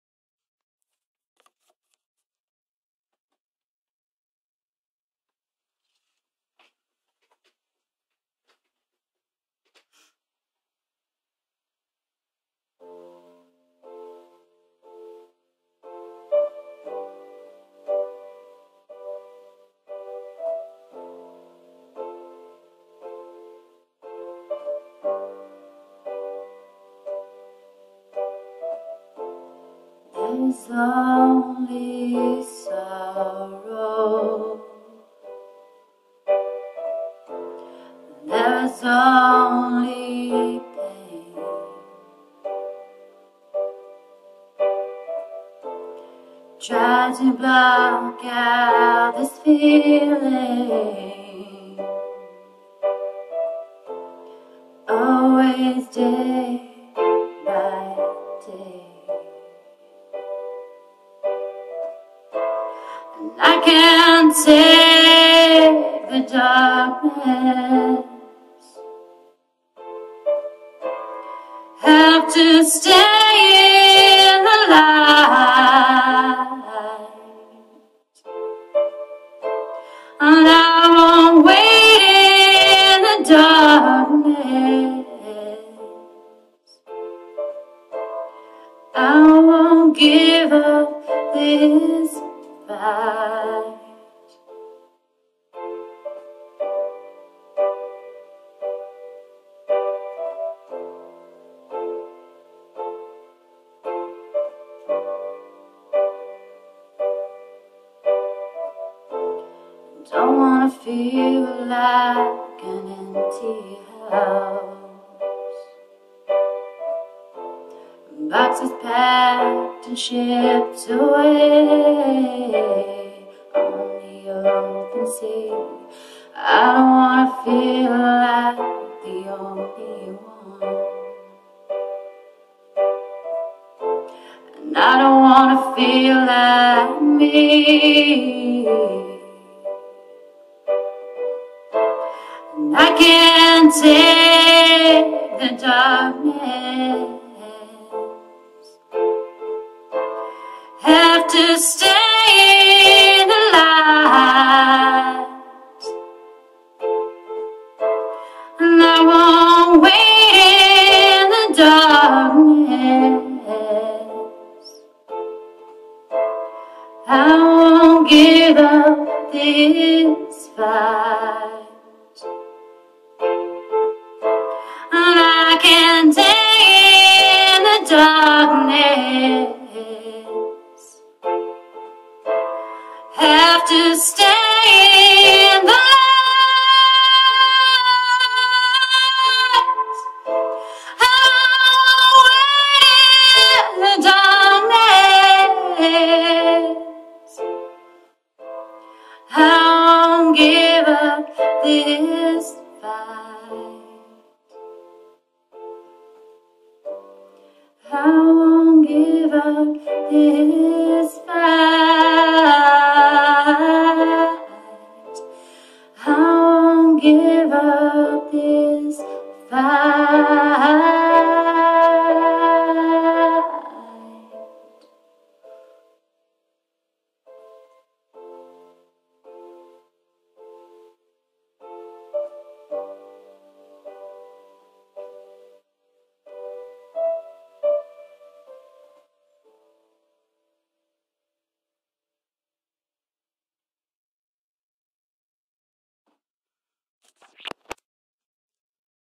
Emma Cherry New Song (a work in progress) !
Hello, long time no see.

So - this song I wrote about an hour ago!

Yep, it's in it's most simple format. the piano is plinky plonky and needs some serious development and the vocal arrangement is getting there.

Yes, I also should have sorted out the sound and put some make-up on ;) and the camera didn't want to focus...but whatever!

It's just good to be writing again, even if it is somewhat depressing. It's been such an emotional year and I guess somethings need to come out and be set free  x